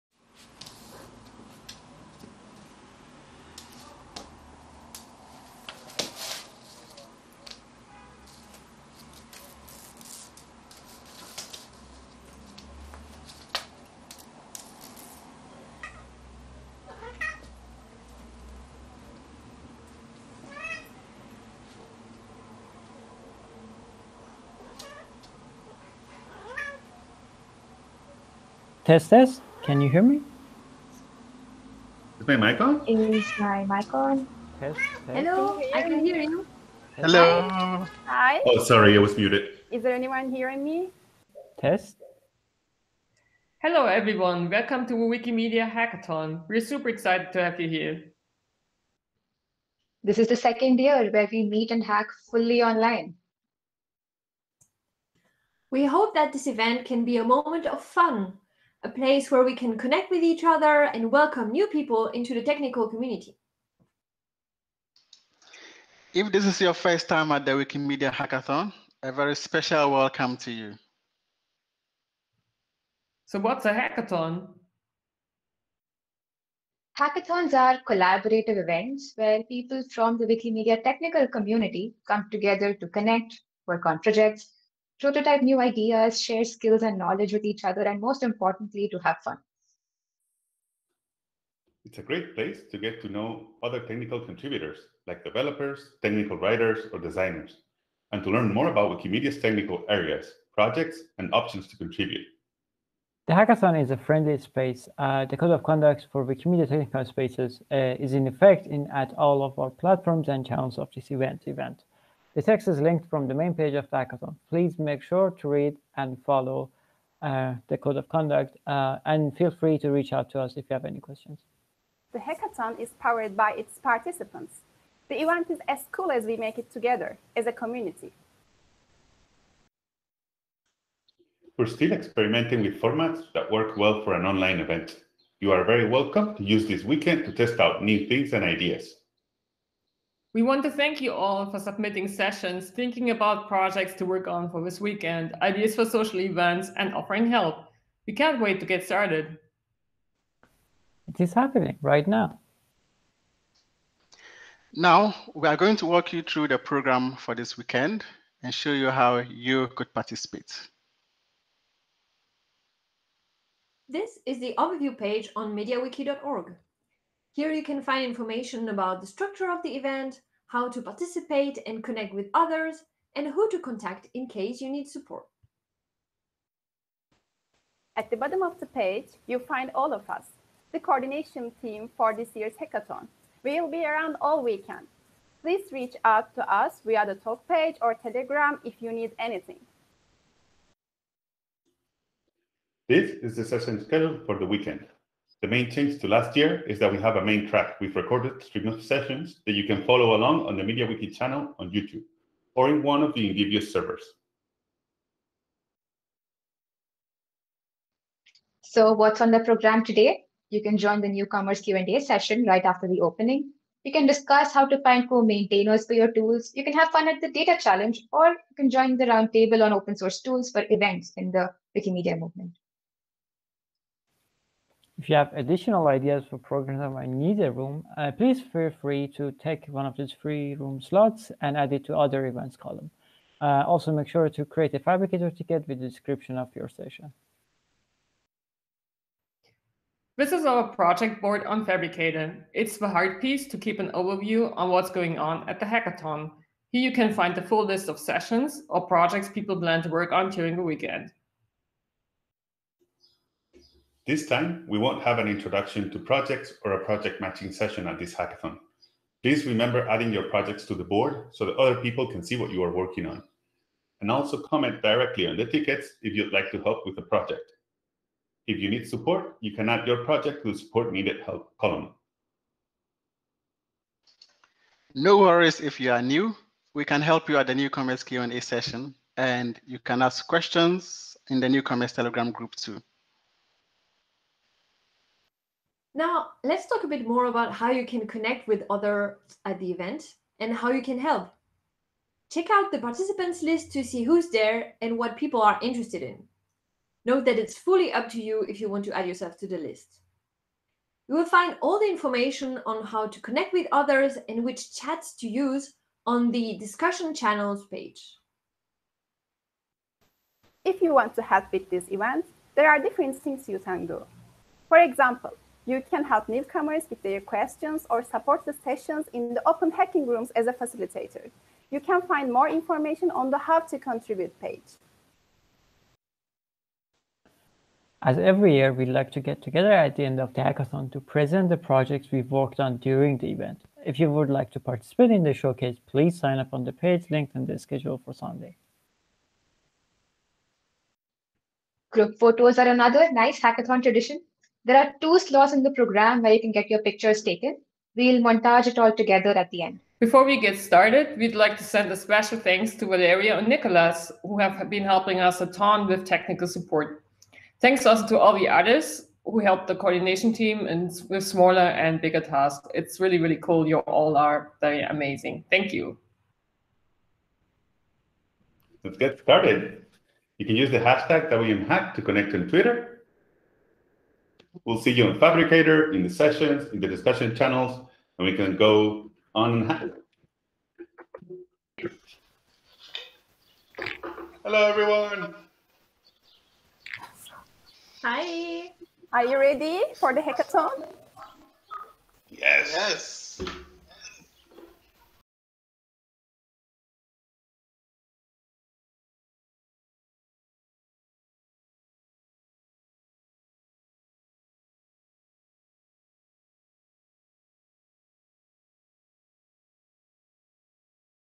Wikimedia Hackathon 2021 - Opening session
Welcome to the remote Wikimedia hackathon 2021!

The Wikimedia Hackathon 2021 will take place as a remote event on May 21-23. This event is open to all people who are interested in discovering the technical aspects of the Wikimedia projects, working on specific tools, features or projects, and socializing with other participants.

for more information and join us!